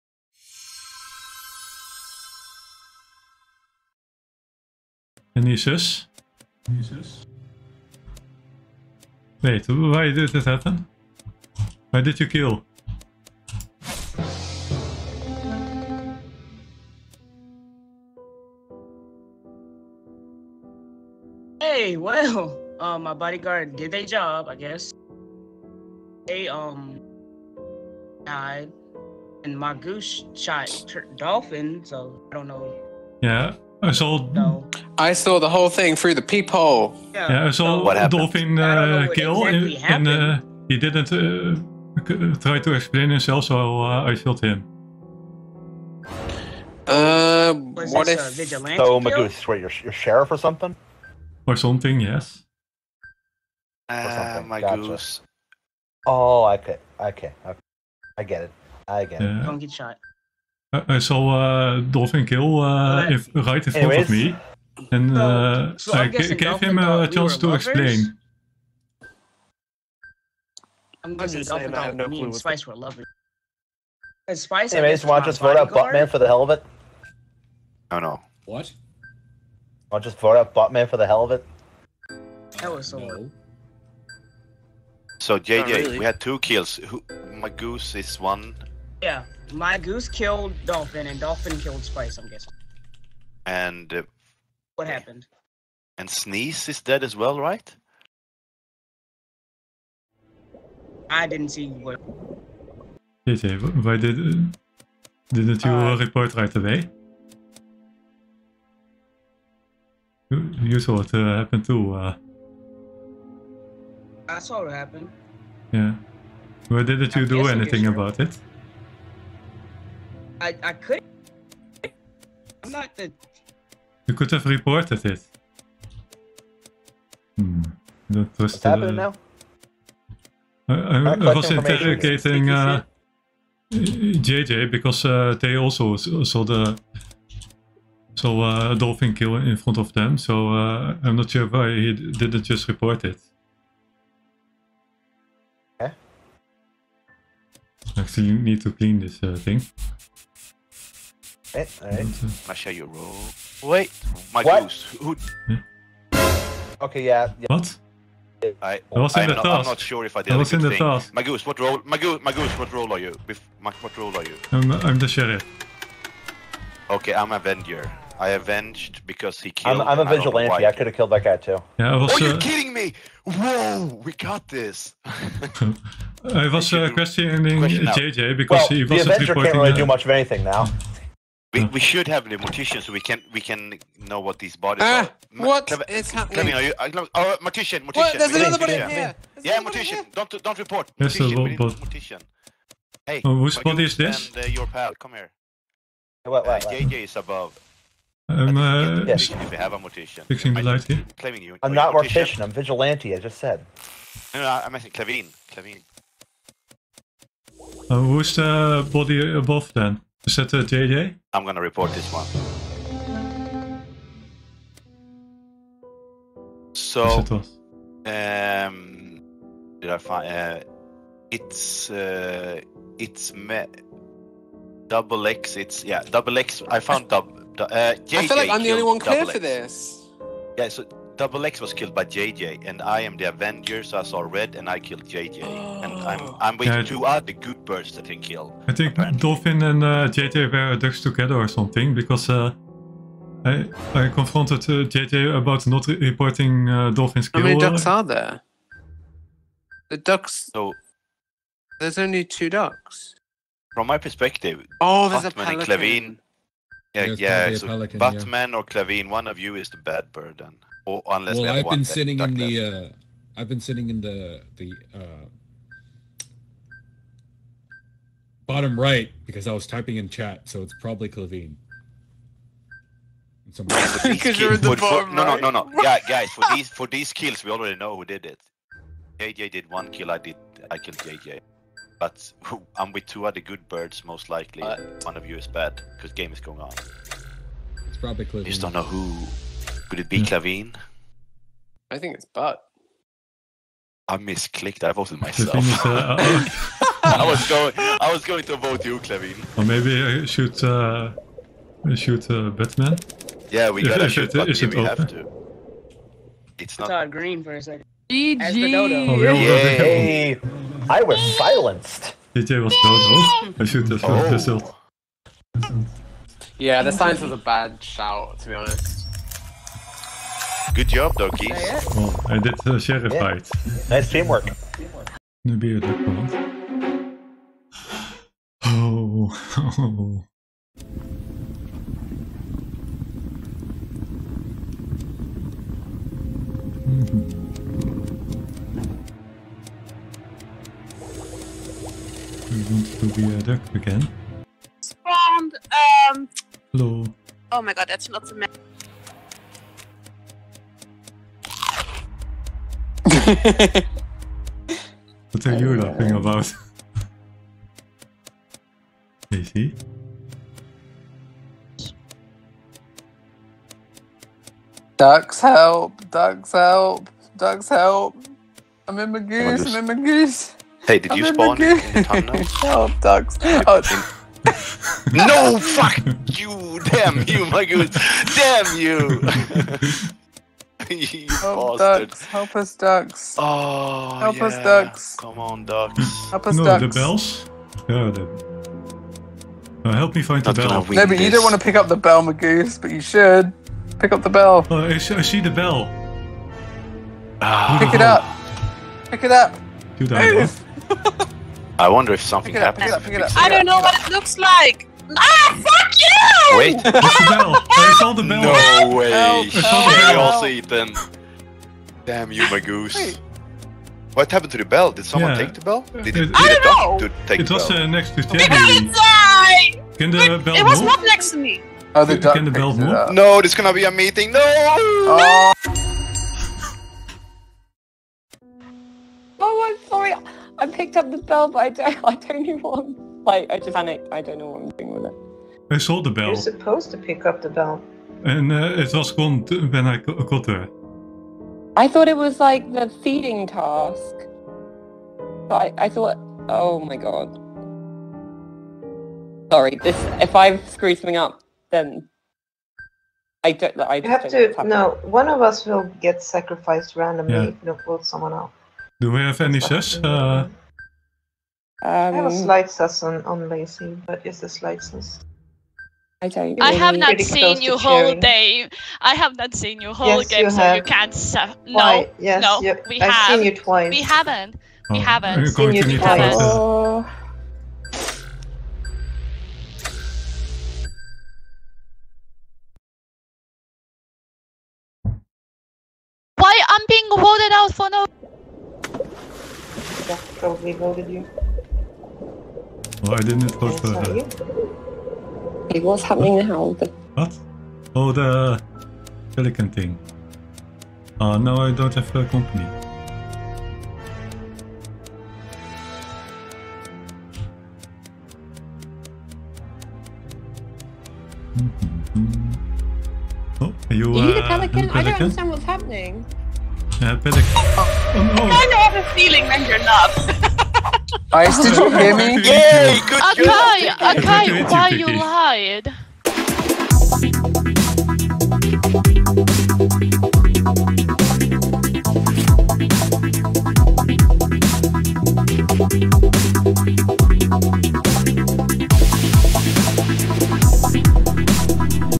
"Any sus?"
The geese with guns team up and I collect birds in my beak as the pelican again.